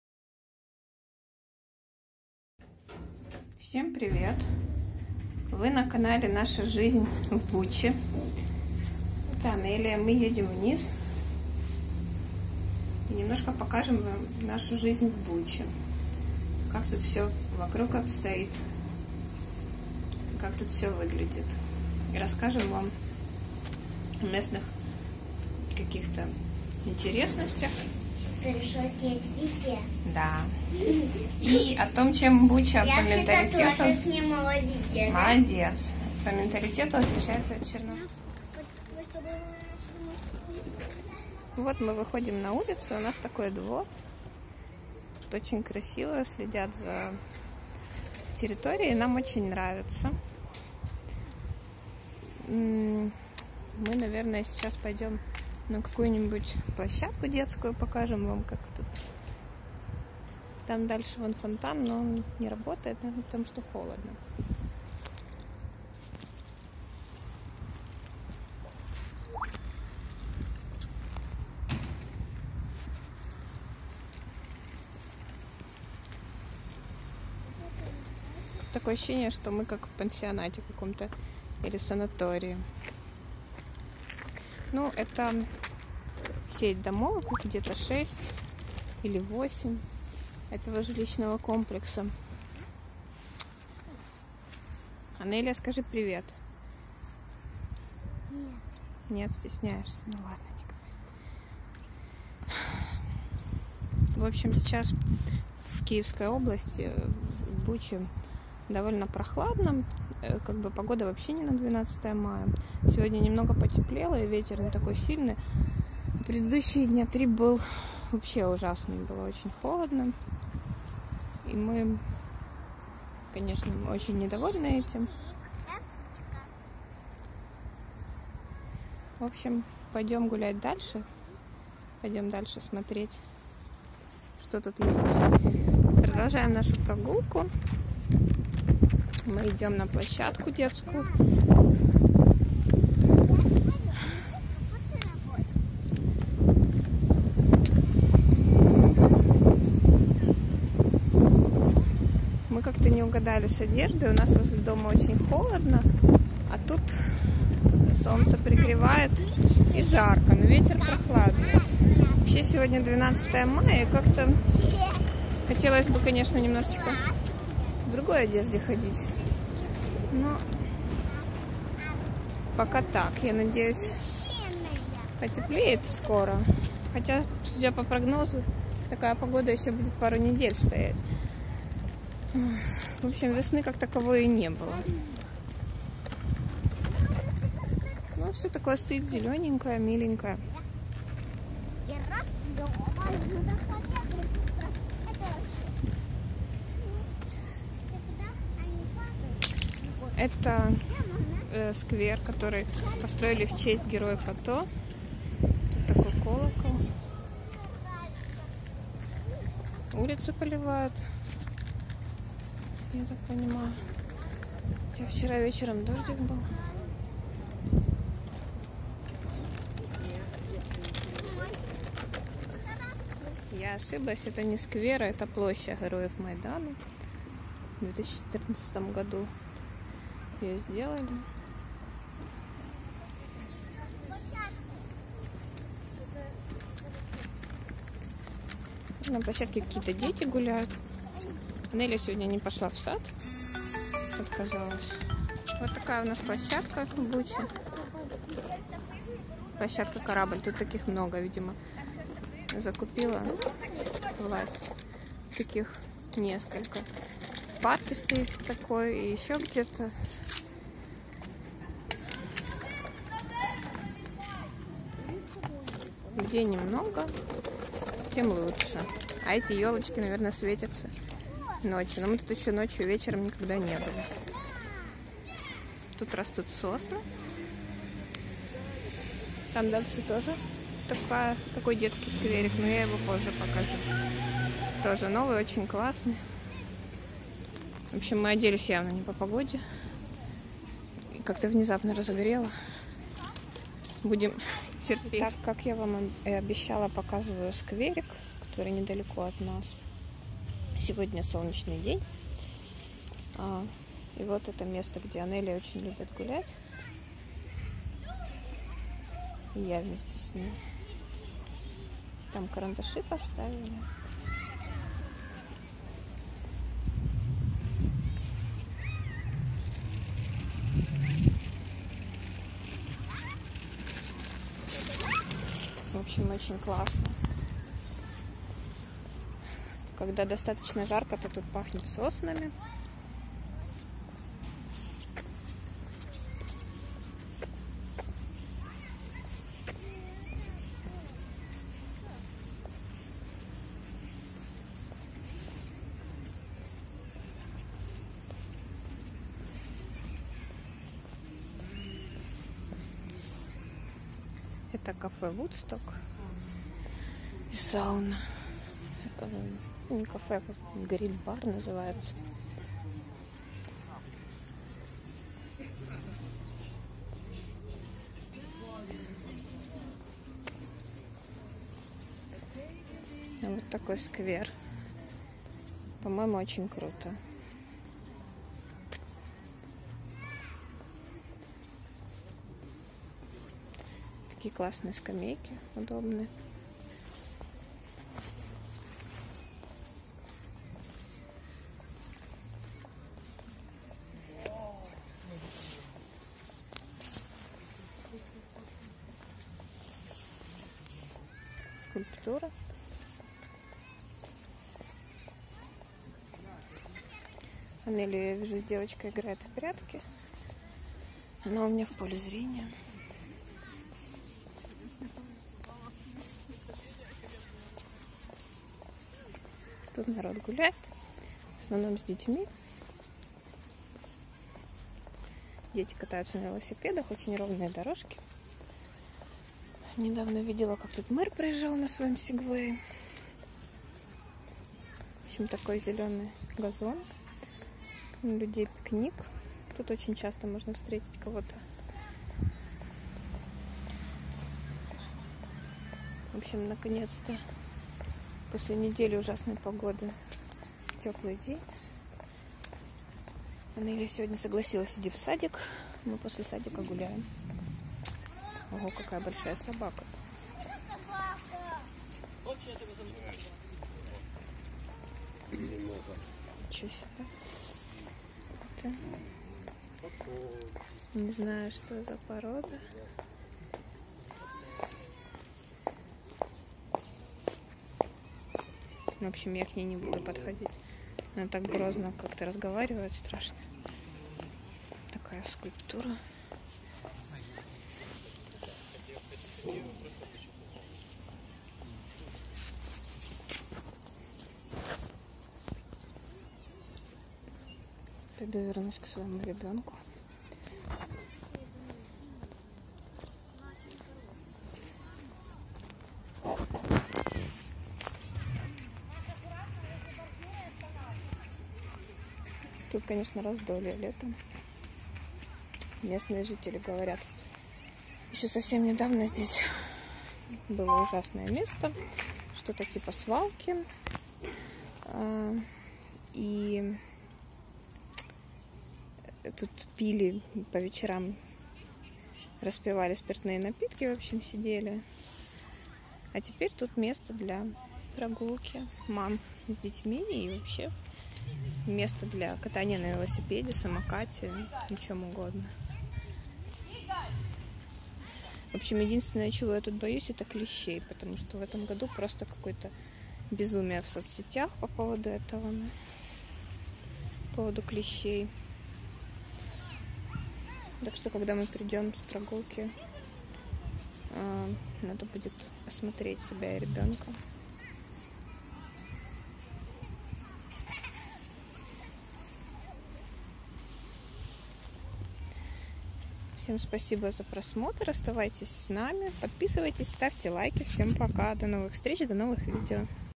Небольшая прогулка по центральной Буче.
Гуляем с дочкой по центральной Буче, решили снять видео, чтобы показать вам, как живется в пригороде Киева. Следующим сделаю видео о различии менталитета жителей Западной, Центральной и Восточной Украины.